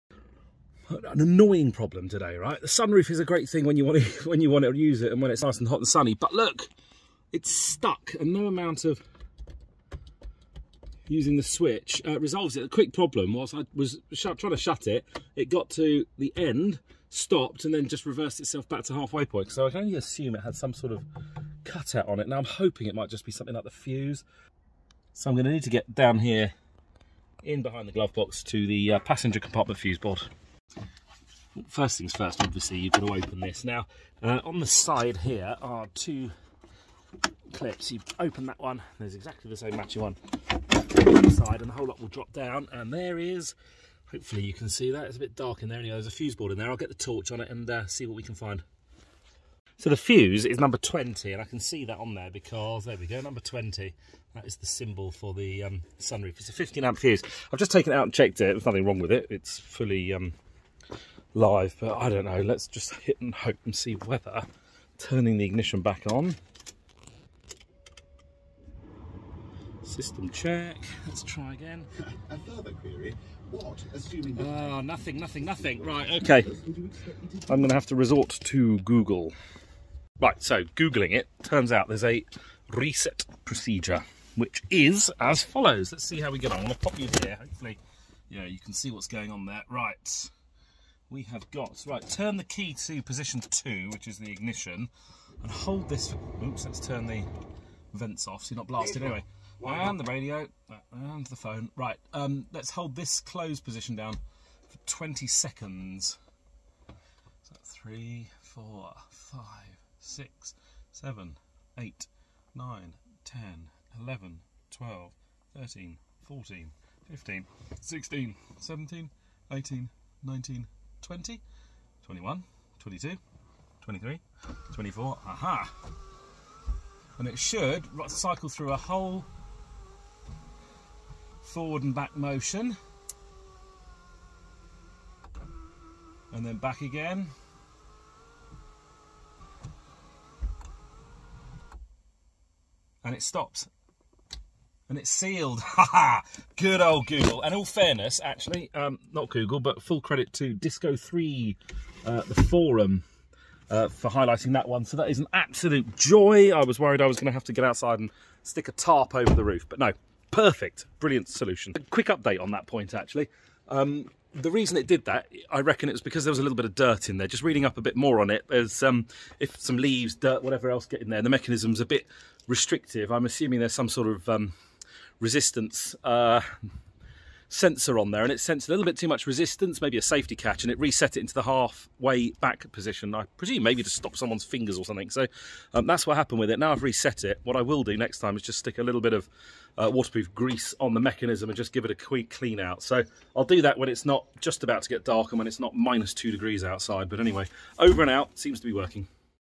Sunroof stuck open? Fixed in 2 minutes at no cost! This time on a Discovery 3 / LR3 Land Rover.
The sunroof on this Land Rover Discovery 3 / LR3 was stuck open in the half-way position and would not close - the switch did nothing.

In this video I checked the fuse first and then did a reset procedure - an easy 25 second job that fixed it.  Bingo!  No cost...